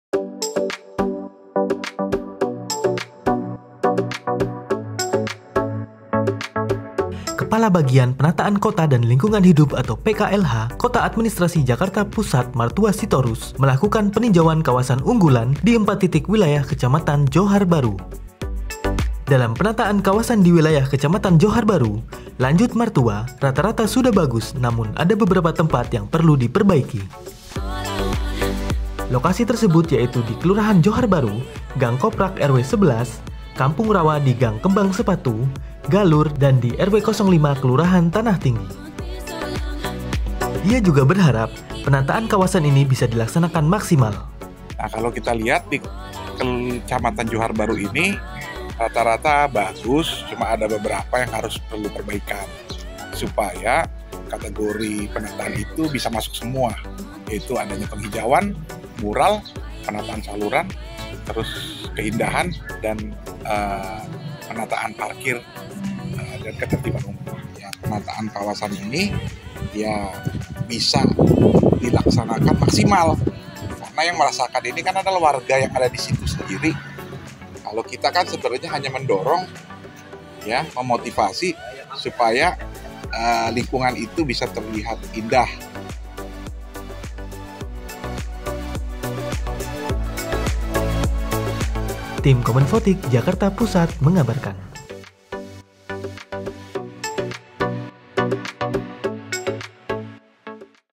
Kabag PKLH Jakpus Tinjau Penataan Kawasan di Empat Titik Kecamatan Johar Baru
Kepala Bagian (Kabag) Penataan Kota dan Lingkungan Hidup (PKLH) Kota Administrasi Jakarta Pusat Martua Sitorus melakukan peninjauan kawasan unggulan di empat titik wilayah Kecamatan Johar Baru, Rabu (27/9).

Pada peninjauan lapangan ini terdapat empat titik yang didatangi, yaitu Kelurahan Johar Baru Gang Koprak RW 11, Kampung Rawa di Gang Sepatu, Galur, dan RW 05 Tanah Tinggi.

Dalam penataan kawasan di wilayah Kecamatan Johar Baru, menurut Martua, rata-rata sudah bagus namun, ada beberapa tempat yang perlu perbaikan yang ditargetkan selesai pada akhir September.

Dalam peninjauan keempat titik tersebut, Martua juga menyarankan untuk melakukan penataan parkir di Jalan Purbaya RW 05 Kelurahan Tanah Tinggi agar bisa tertib dan dari jajaran kelurahan disarankan untuk melaksanakan pendekatan ke warga.

Penulis Naskah : Andreas Pamakayo
Kameramen : Rio Cornelianto
Editor Video : Dwi Arif S
Pengisi Suara : Dwi Arif S